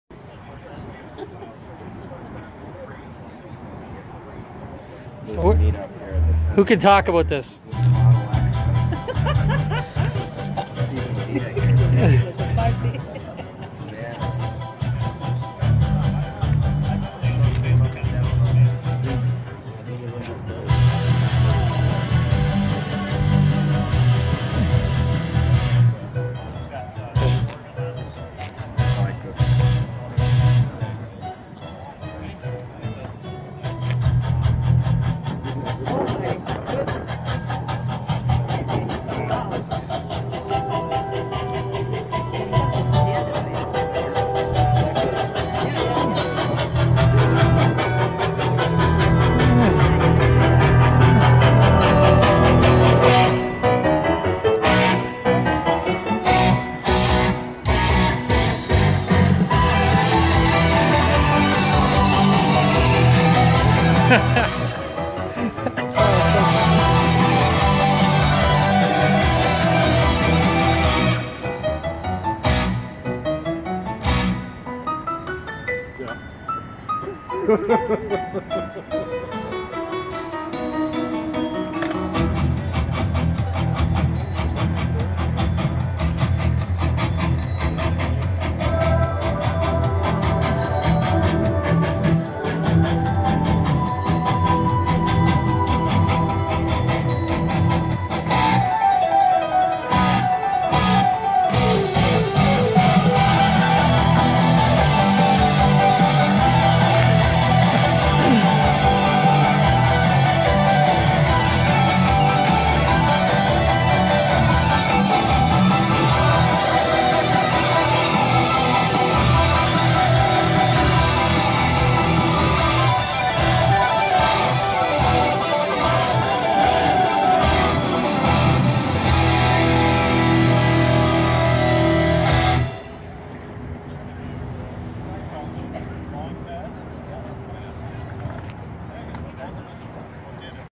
Tesla Model X Holiday Light Show @ P.Eng ONT Niagara & GHEVA meeting
Fraser Horn's model X takes centre stage in the parking lot of the Casablanca Winery Inn Grimsby. A great joint meeting of Professional Engineers and Golden Horsheshoe Electric Vehicle Association (GHEVA) members with 62 in attendance.